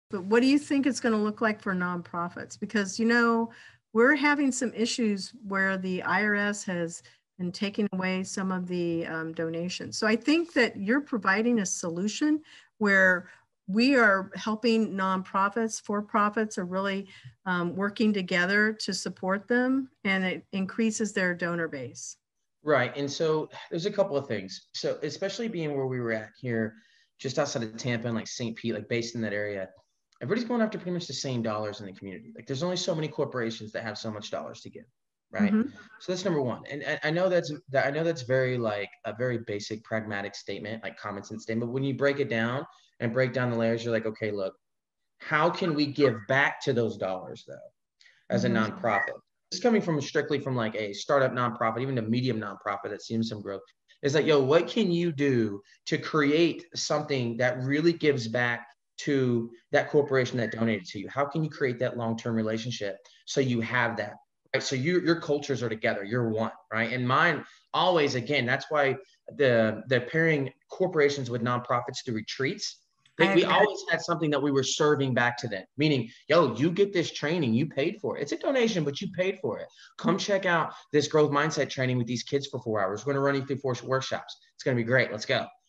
S4 Ep. 133 Beau Blouin Snippet 3
We all need to feel connected somewhere. Beau Blouin speaks on his business, Connective Human, and how it helps people make those connections.

Intern Pursuit’s intelligent intern platform makes it easy for employers and students to find their perfect match.